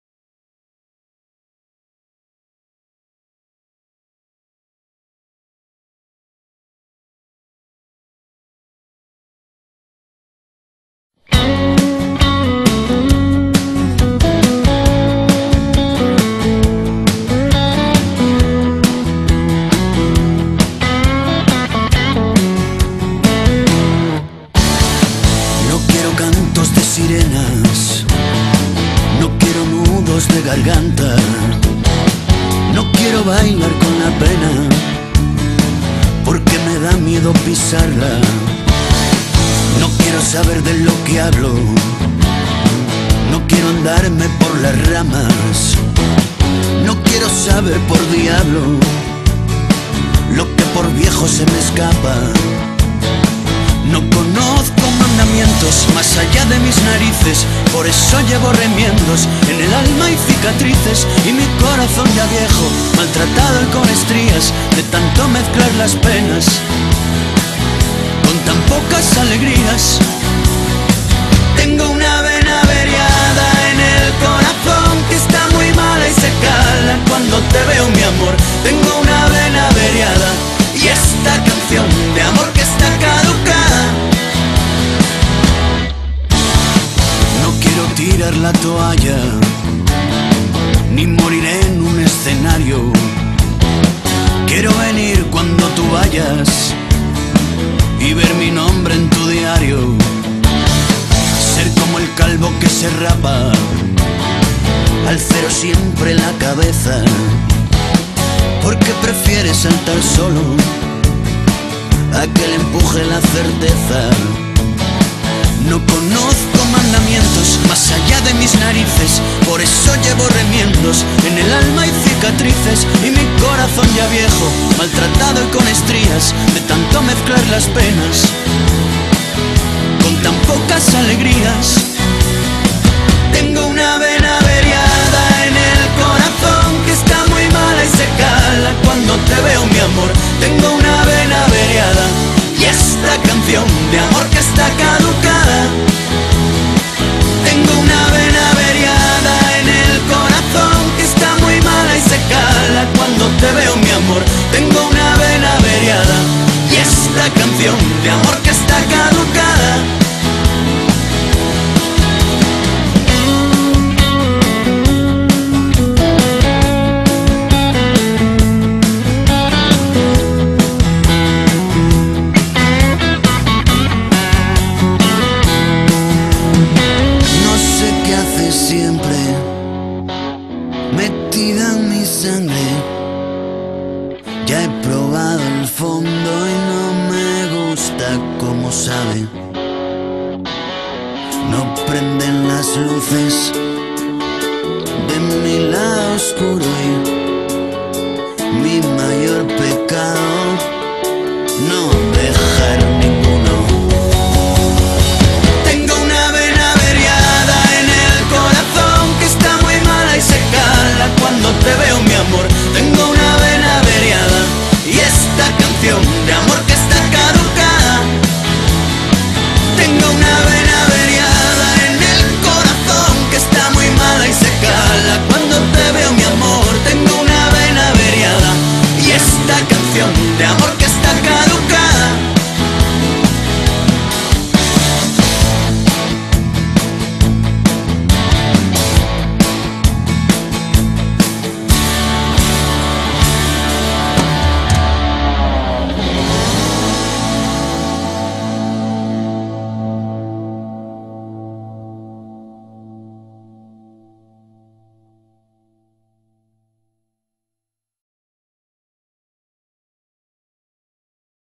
I Campus Natural Cycling.mpg
Homenaje a tod@s y cada un@, de l@s que a traves de vivencias personales, convivencias, ciclismo indoor, y pedaladas por un buen comun, hicieron que un Campus, fuese toda un fin de semana de hermanamiento, de conocimiento, de amistad. Homenaje a Miguel Angel Pastor, y Sandra Santamaria, por crear un campus para el conocimiento profesional, y personal, de tod@s los asistentes, Gracias a tod@s, llevais por bandera el ser buena gente, Gracias Miguel, Gracias Sandra, no sabeis el bien que nos habeis hecho, besos